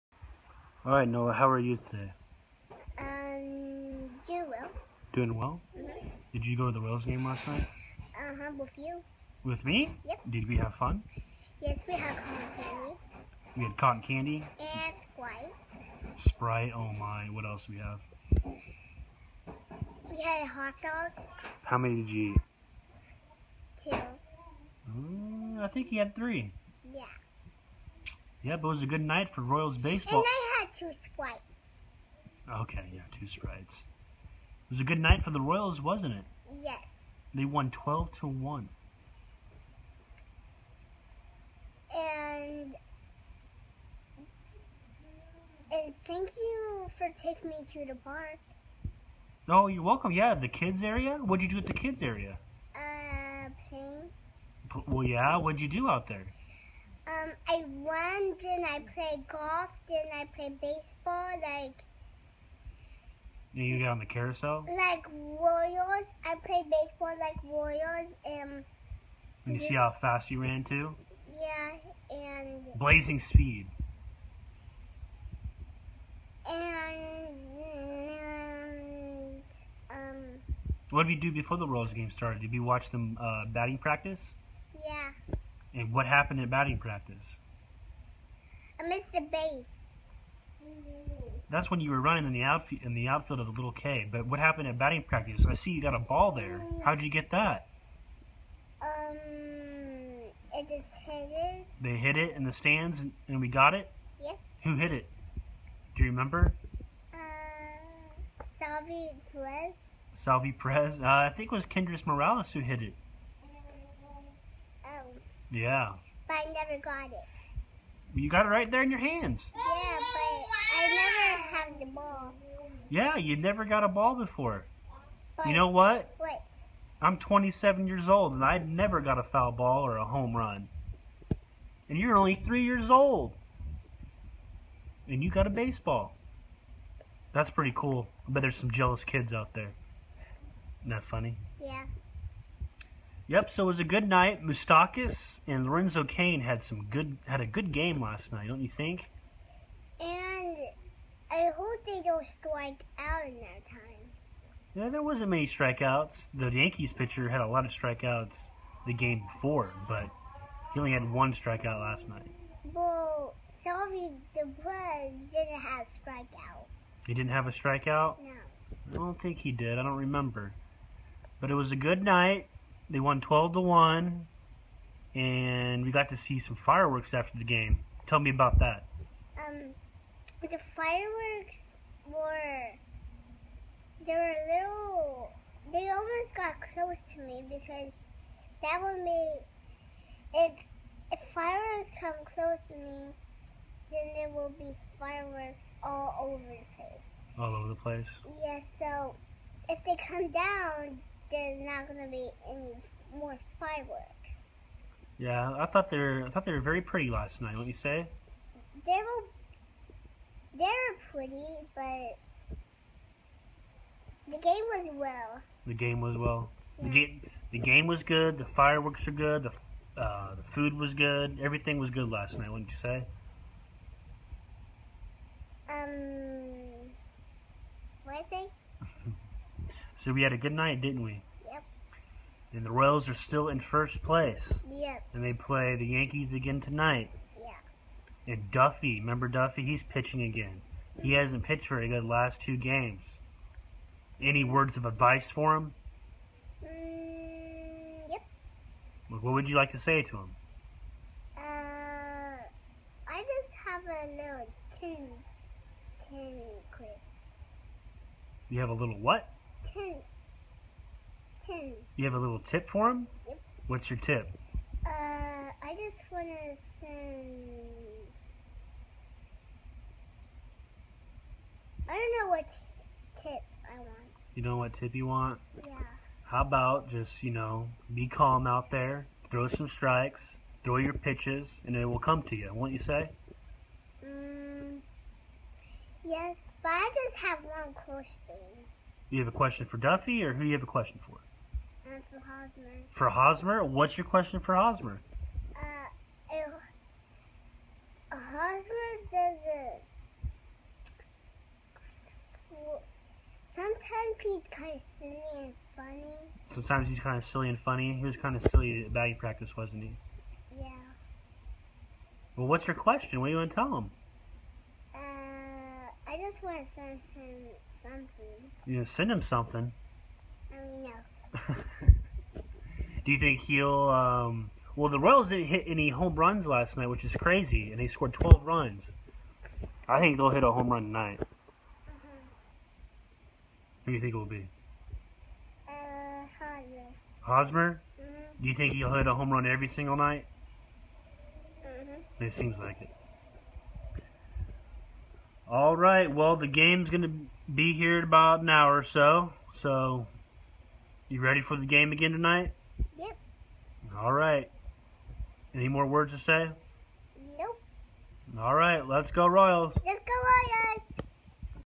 Royals Talk With Nola (5/16/15) Caught a home run in batting practice!
Talking Royals with Nola. Yankees series.